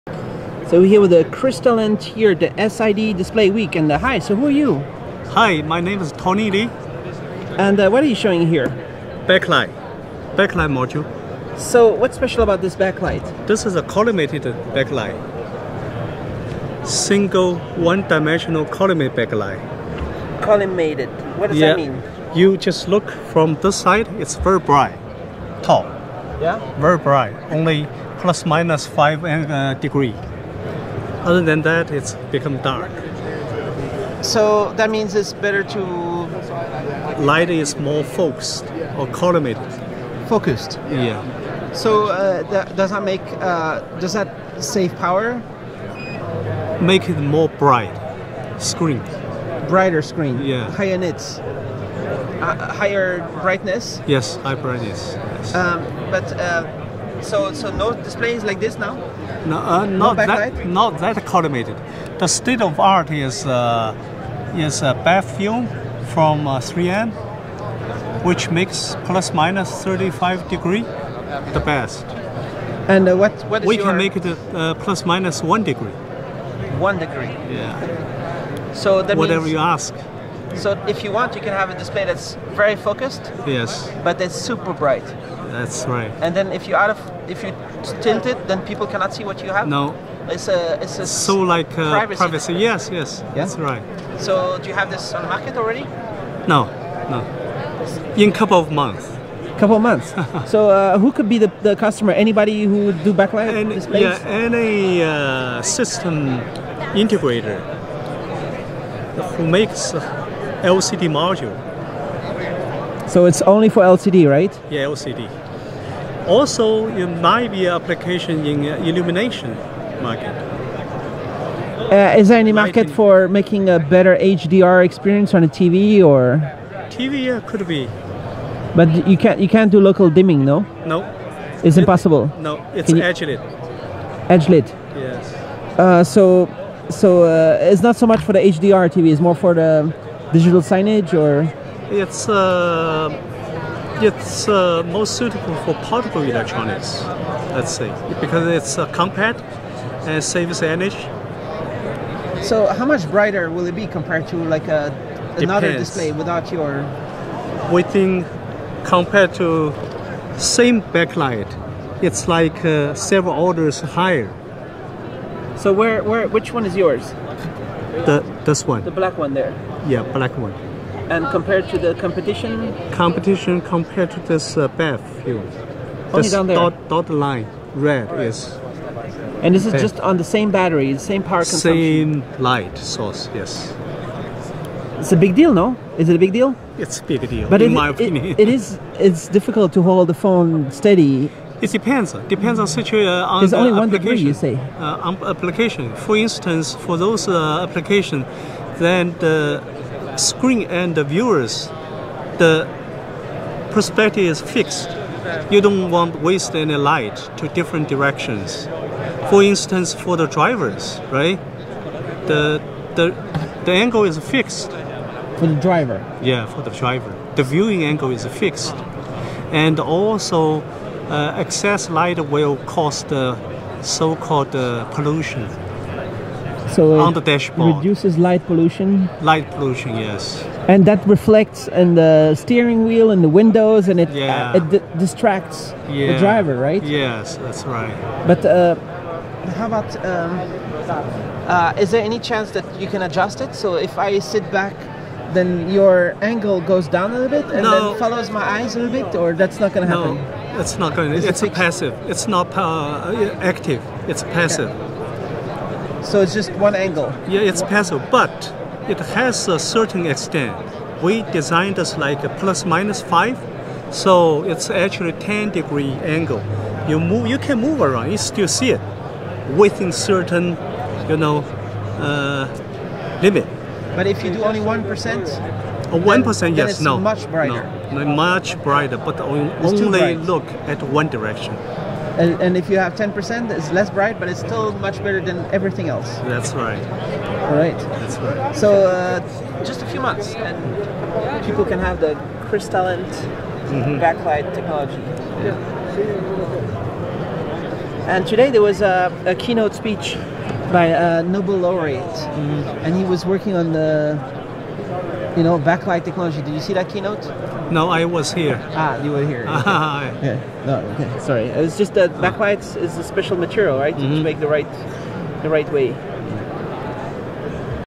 Suzhou Crystalent edge-lit, single layer collimated roll-to-roll nano backlight
Suzhou Crystalent demonstrated its edge-lit, single layer backlight or forelight module and a collimated backlight module based on compound roll-to-roll nanofabrication technology. The former is transparent, flexible, compact, lightweight and low-cost with a thickness of less than 0.5mm; while the latter is a four-layer flexible module with a thickness of less than 1.5mm. The unit can be laminated onto an LCD panel as either backlight or forelight for transmissive or reflective applications respectively; while the latter may serve as a standard platform, combining with various diffusers to result in any desired viewer application.

Filmed at the I-Zone demo and prototype area at SID Display Week, the world's largest and best exhibition for electronic information display technology.

Every year, dozens of applicants submit their pre-market and emerging products to compete for a free booth where they can share their inventions with buyers, manufacturers, potential partners, industry leaders and thousands of attendees.